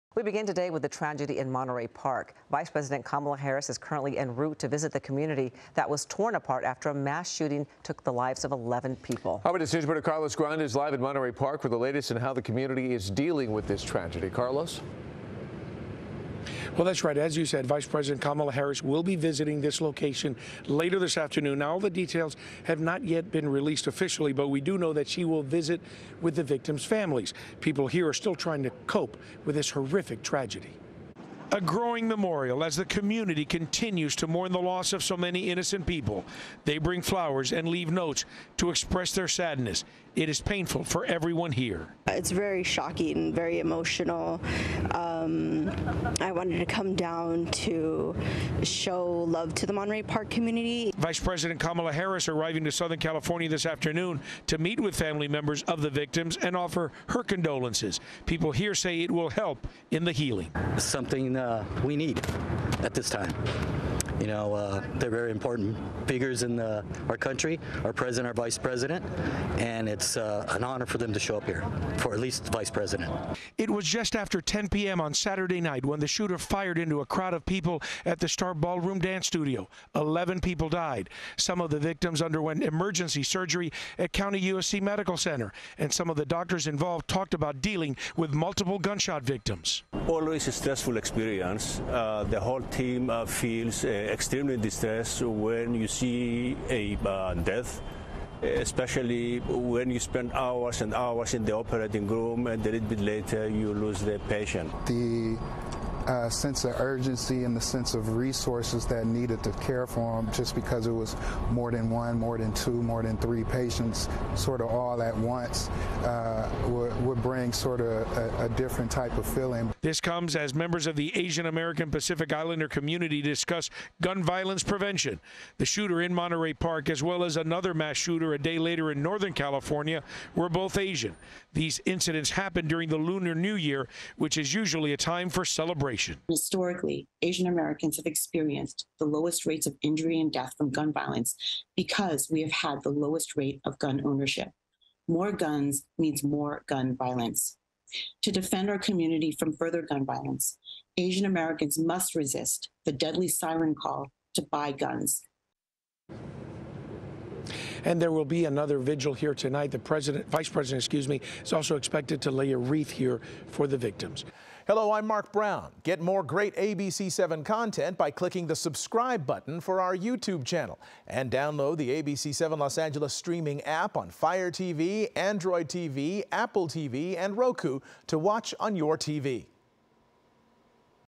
Surgeons who treated Monterey Park shooting victims describe harrowing scene at hospital
The Monterey Park shooting victims underwent emergency surgery at LAC+USC Medical Center. In the video above, some of the doctors involved talked about dealing with multiple gun shot victims. "The whole team feels extremely distressed when you see a death, especially when you spend hours and hours in the operating room and then you lose the patient."